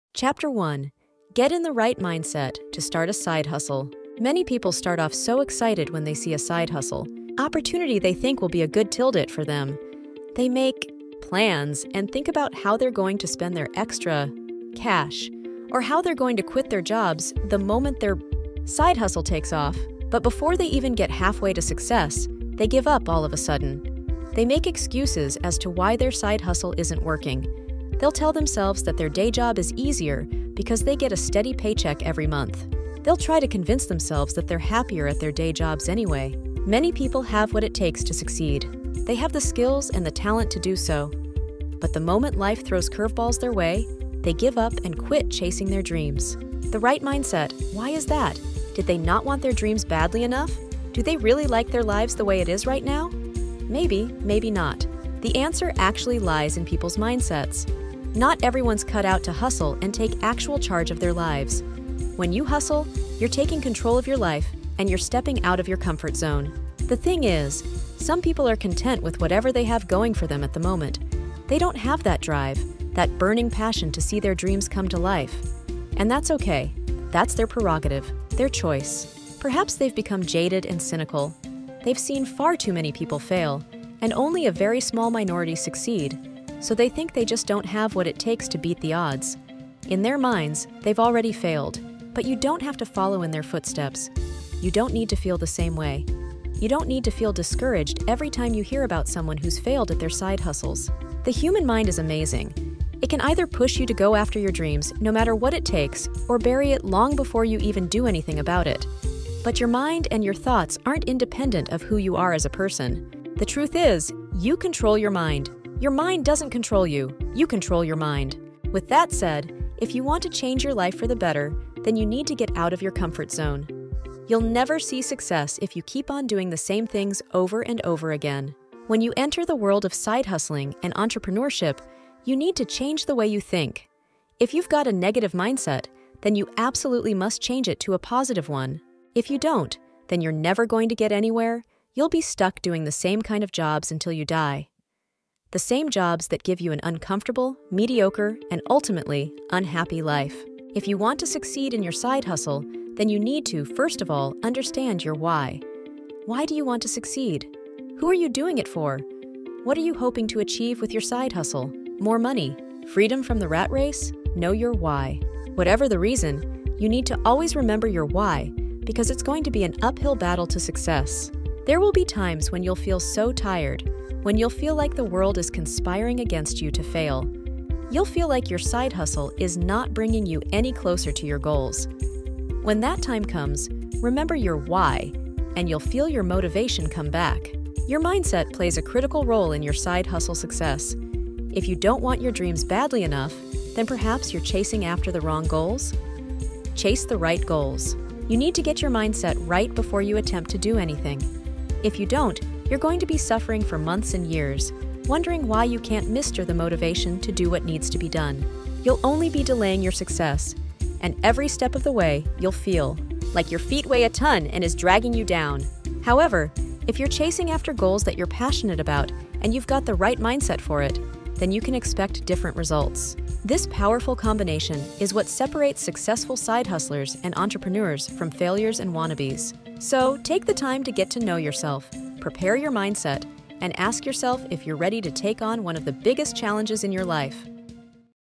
Side Hustles Done Right - vBook | Chapter 1/12
Chapter 1 - Get In The Right Mindset To Start A Side Hustle
From my first vBook "Side Hustles Done Right"

It is a guide that covers starting and scaling a profitable side hustle, balancing it with your main job, time management, and choosing the right hustle. It includes marketing strategies, burnout prevention tips, and steps to success for achieving financial freedom and your desired lifestyle.